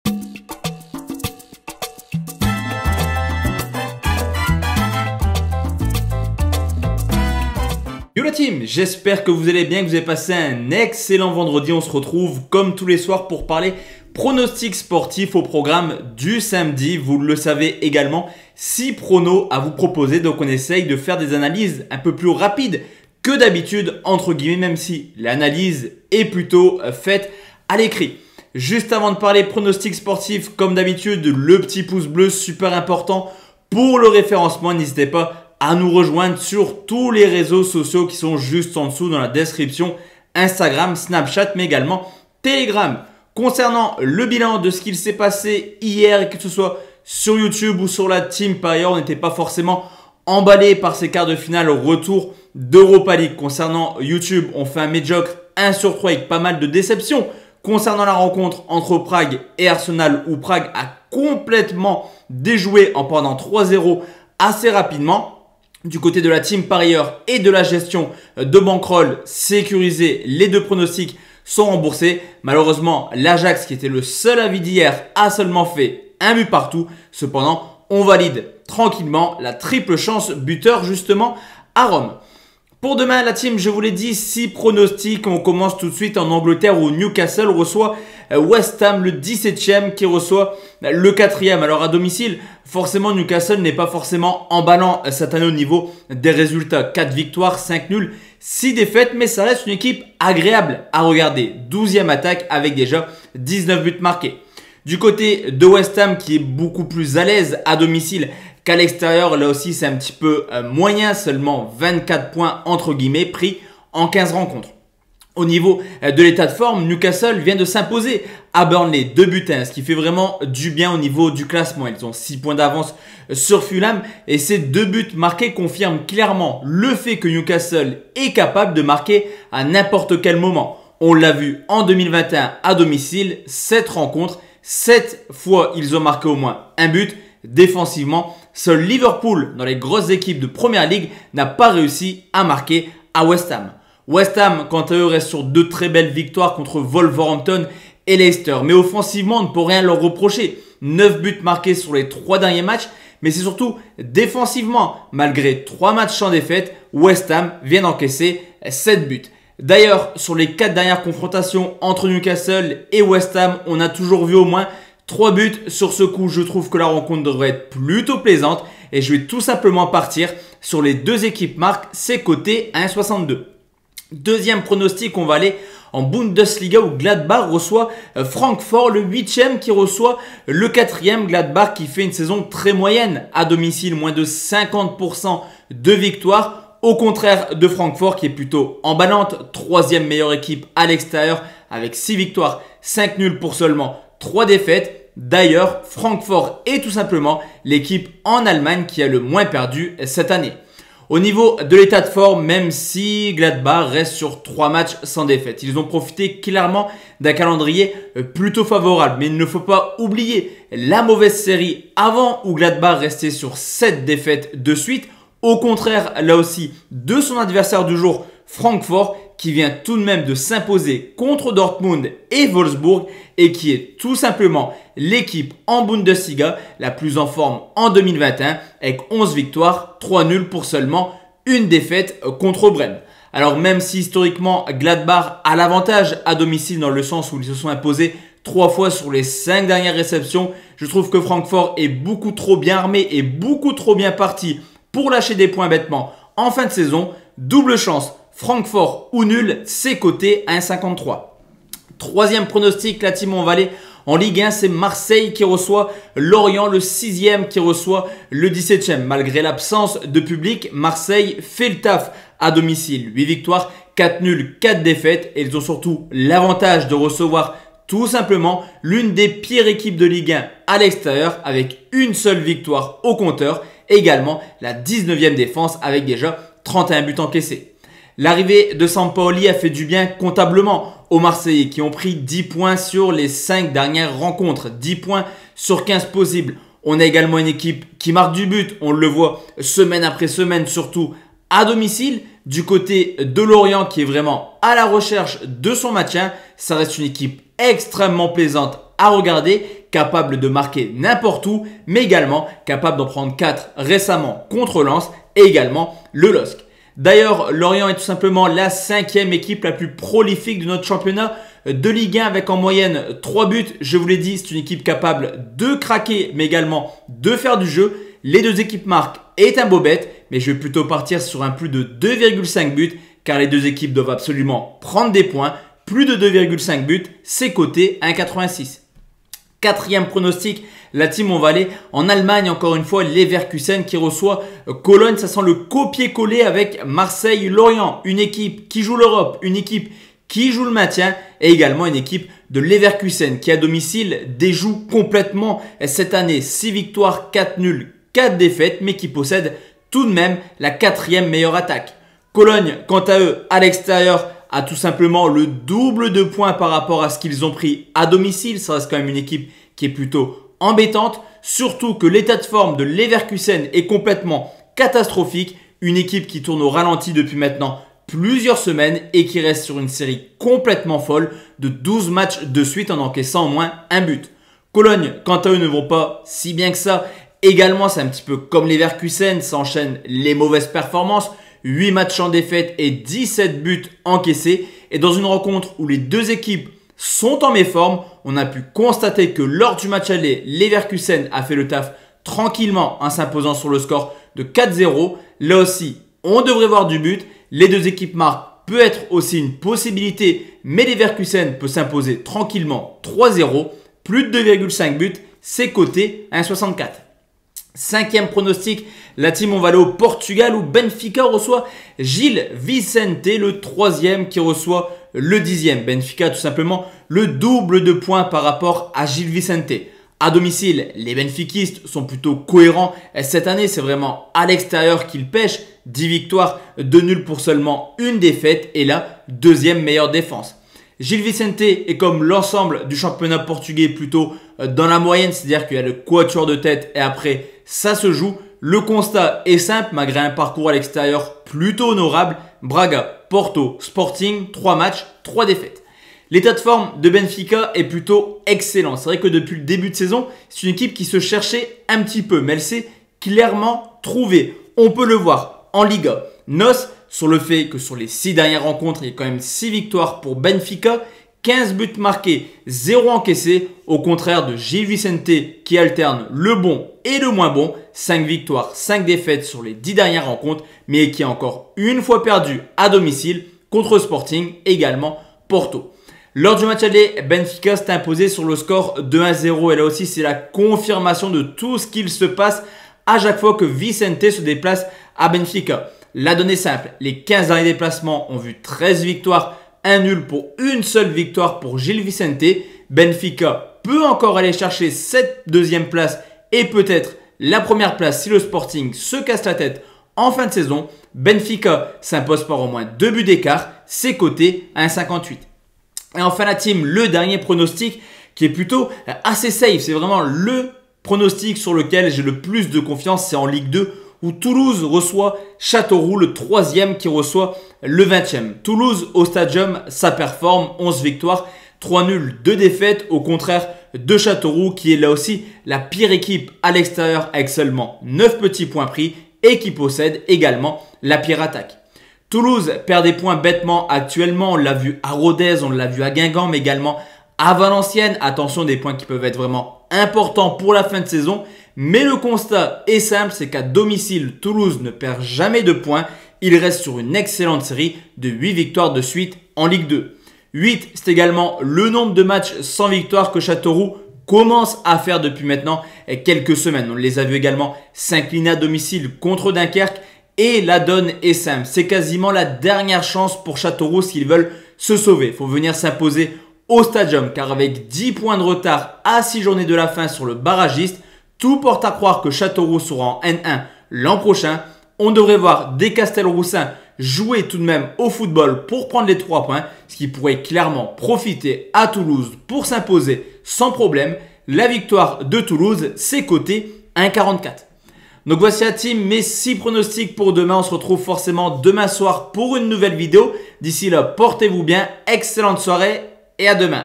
PRONOSTICS DU 17 AVRIL / MARSEILLE - LORIENT / GLADBACH - FRANCFORT / NEWCASTLE - WEST HAM
Yooo la Team !